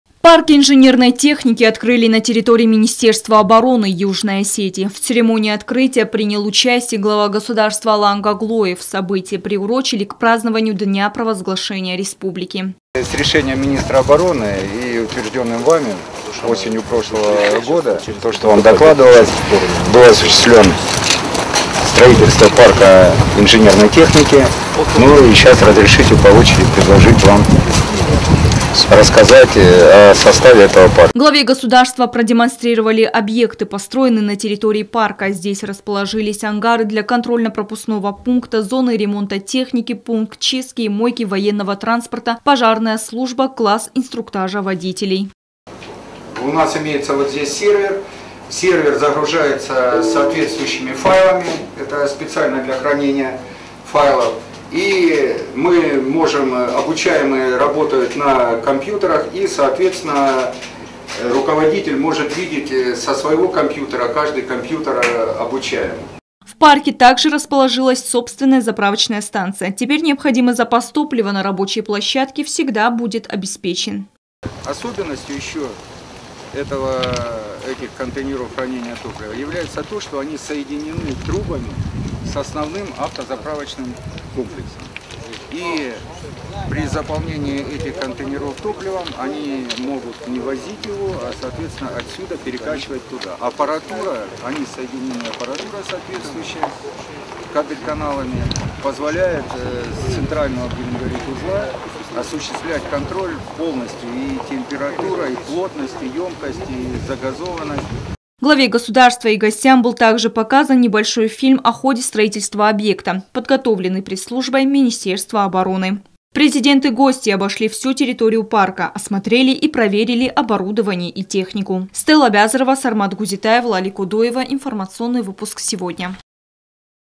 Президент Алан Гаглоев принял участие в церемонии открытия парка инженерной техники
Объект открыли на территории расположения Министерства обороны Республики Южная Осетия. Мероприятие было приурочено к празднованию 33-й годовщины провозглашения Республики Южная Осетия.